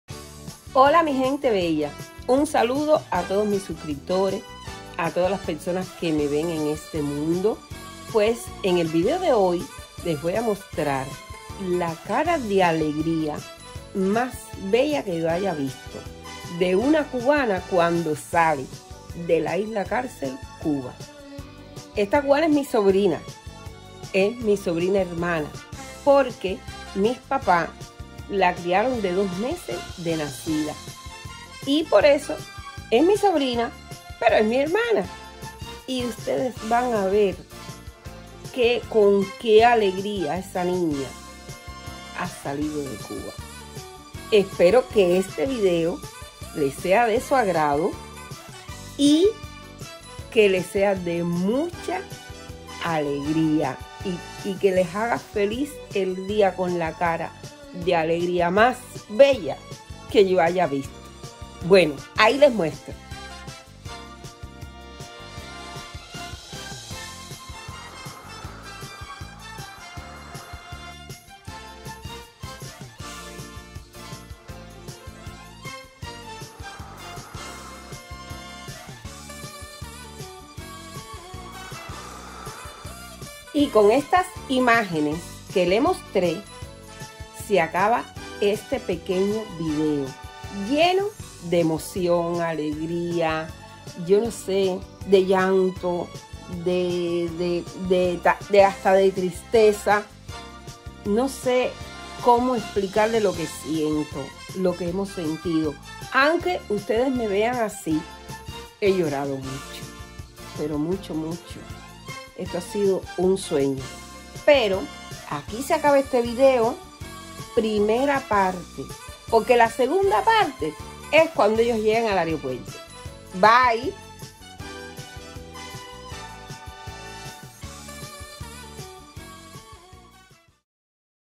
Cubana Super Felíz por salir de CUBA.
Cuba, cuba, cubana,
Este video que se titula:Cubana Super Felíz por salir de CUBA. cuba. Este video trata, sobre la salida de mi sobrina del país Cuba. Esto es la cara más linda de alegría que yo halla visto en mi vida. Estamos super felíz, contentos, alegres. Espero como siempre que el video sea de su agrado y que lo disfruten 👍💕😊. Para que youtube me lo recomienda y llegue a más audiencia.👍😊💕.